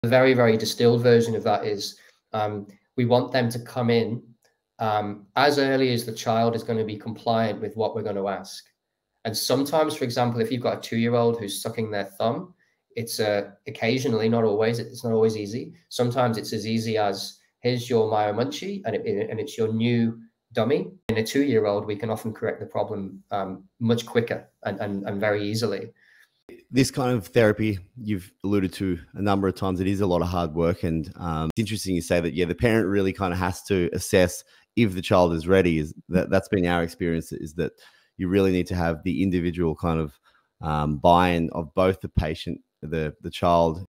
Child Thumb Sucking & Speech Success: Dr. Hanson's Dental Care Guide | Dr Steven Lin
Disclose the power of early dental interventions with Dr. Dan Hanson's expert advice on pediatric dental health. Learn how tools like the MyoMunchie can correct thumb-sucking and speech issues, setting the foundation for your child's oral health and speech development. A must-watch for parents seeking effective solutions.

Simplicity is key. Sometimes, swapping a thumb for a MyoMunchie can quickly and easily correct habits that might later lead to crooked teeth. Straightening out these issues early can lead to significant long-term benefits, from better breathing to proper jaw development. 😁👌

A parent's assessment of their child's readiness plays a pivotal role in the success of these interventions. The commitment from both parent and child is essential for the journey toward a healthier smile and overall well-being. 💪👪

For a daily dose of pediatric dental health wisdom, follow @DrDanHanson on Instagram! 📸